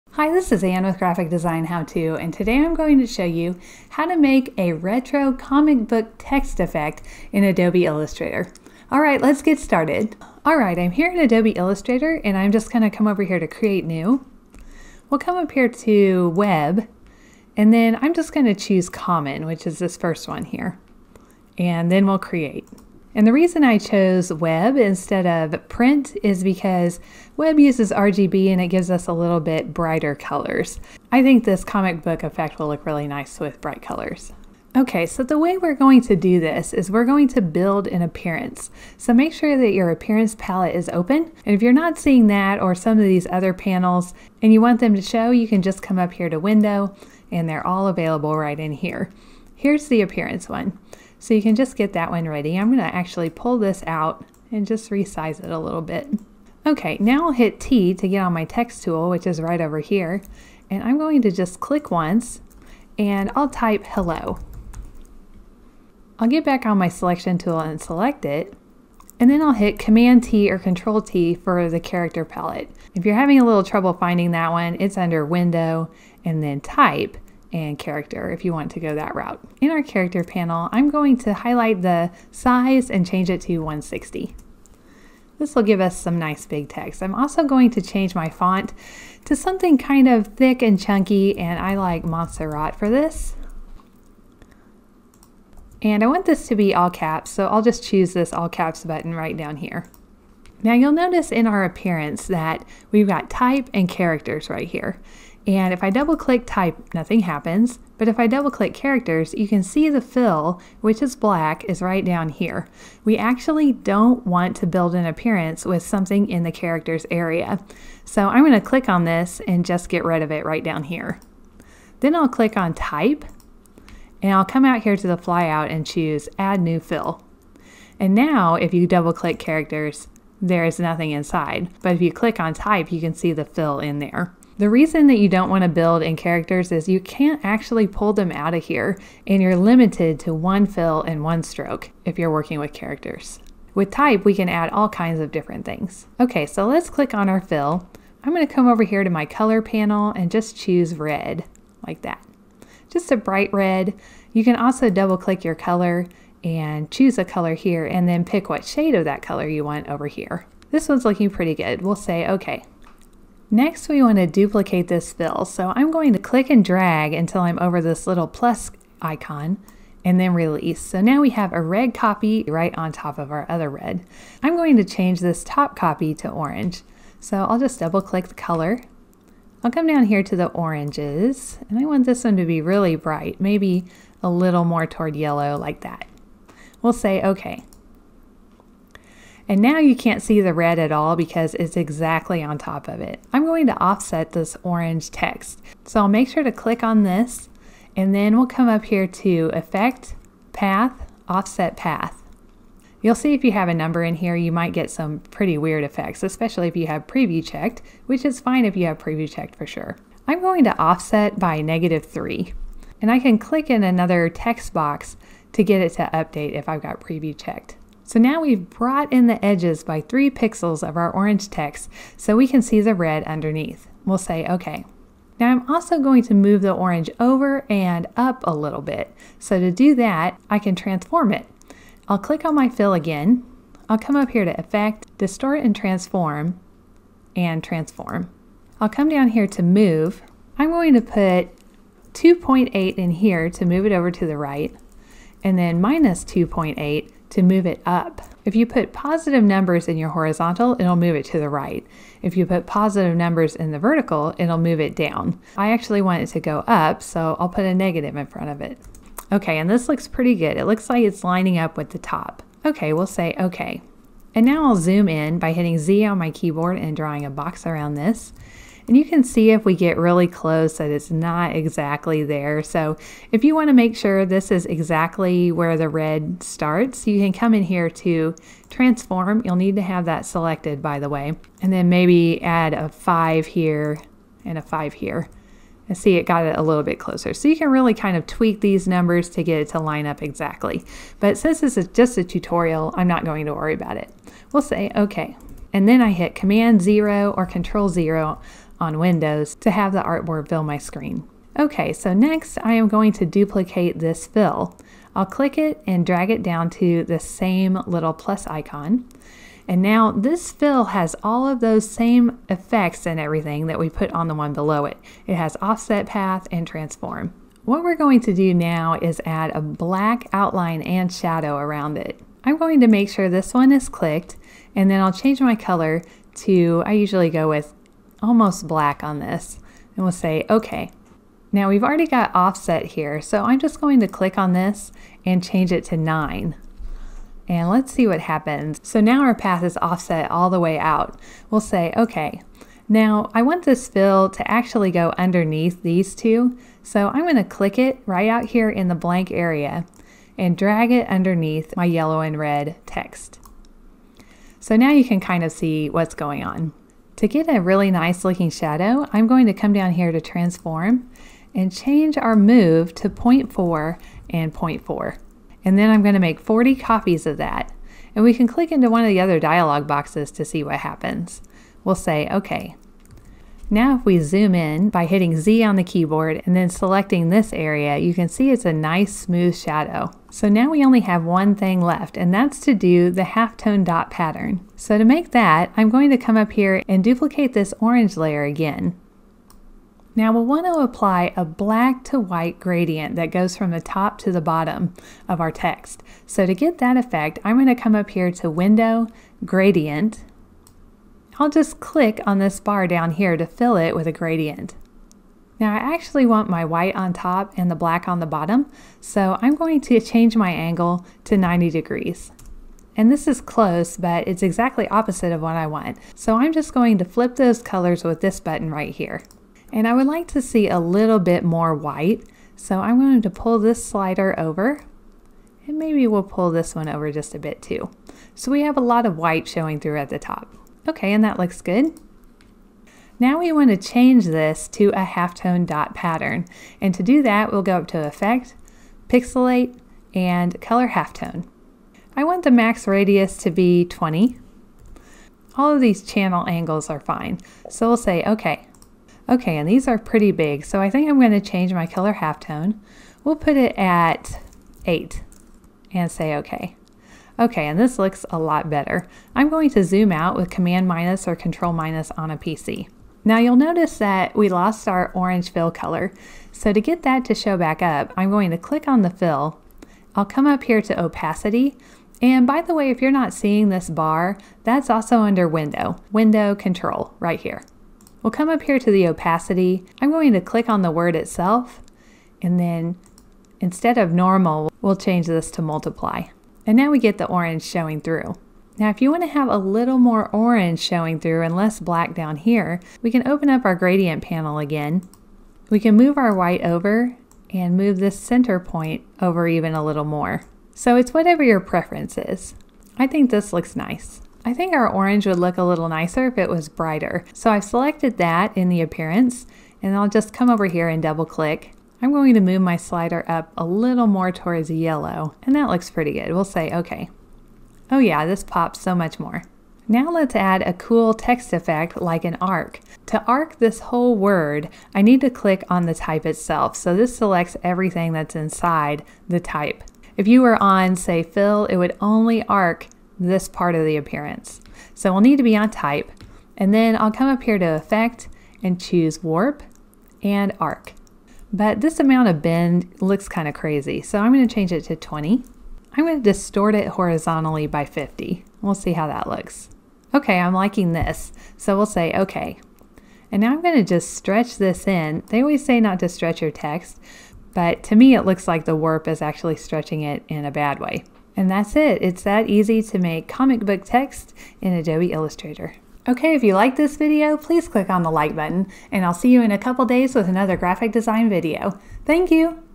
How to make a retro comic book text effect in Adobe Illustrator
FYI: The words POW! and WHAMMO! look really cool with this comic book text effect - learn how to create it yourself in Adobe Illustrator!